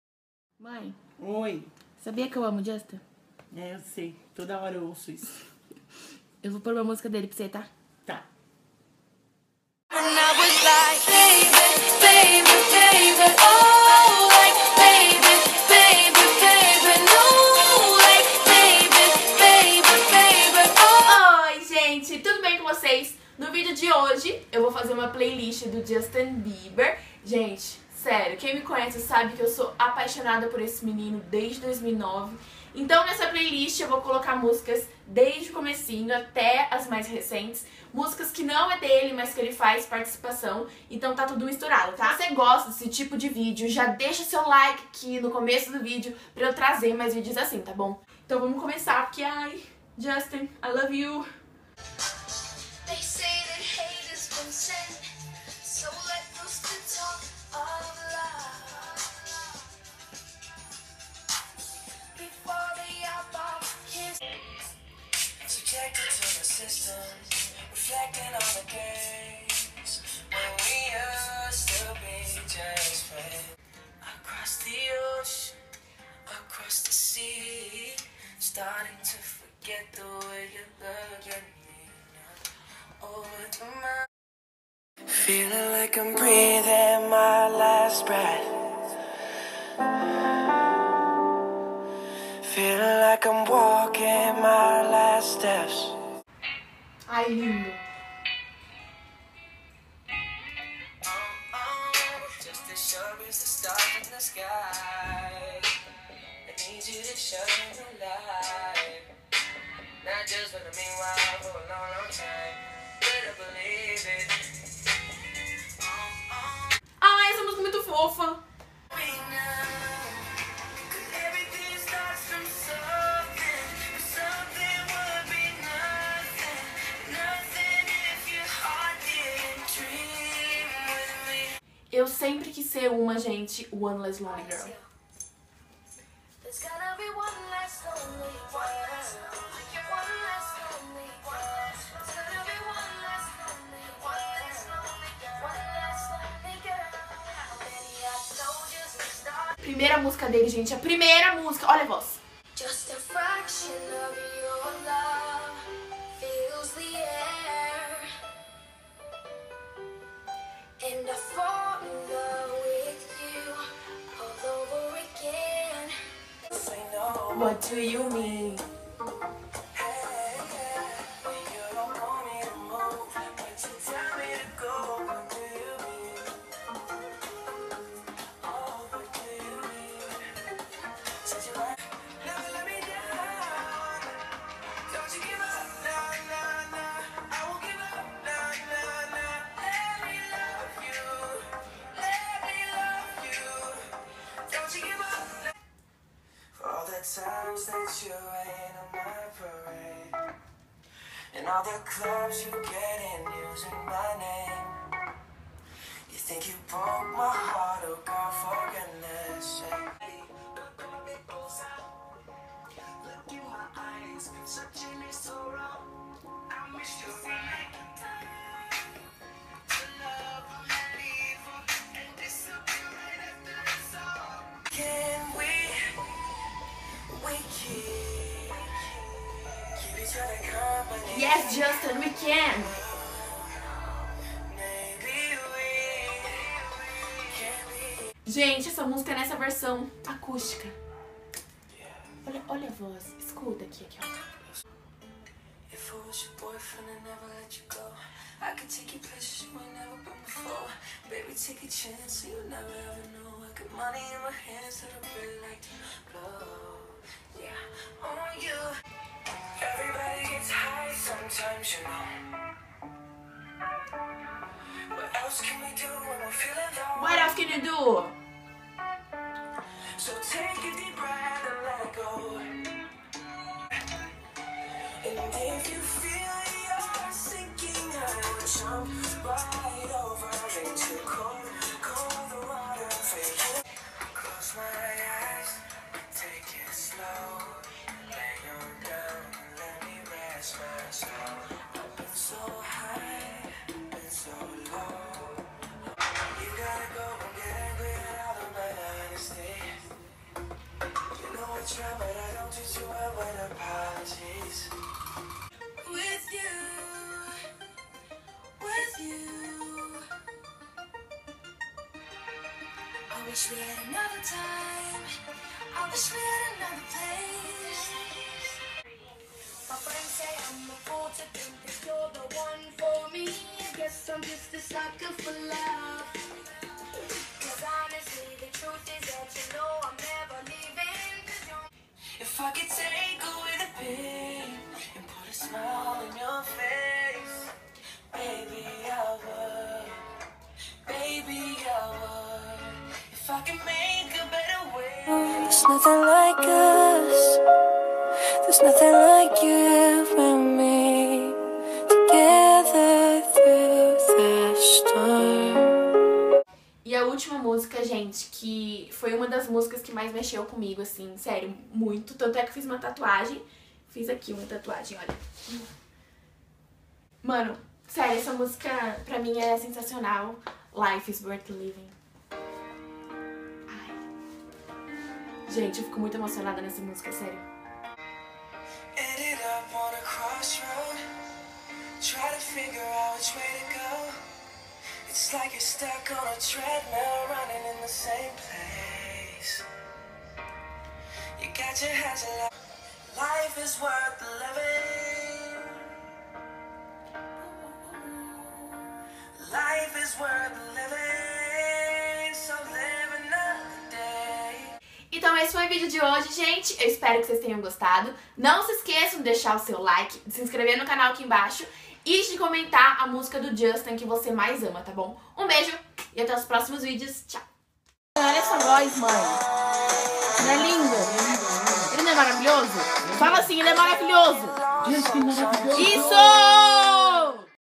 MINHA PLAYLIST JUSTIN BIEBER
Oiiiii gente. Hoje decidi fazer uma playlist de um cantor que eu amo muuuuuito, JB. Espero que gostem. Beijos, fiquem com Deus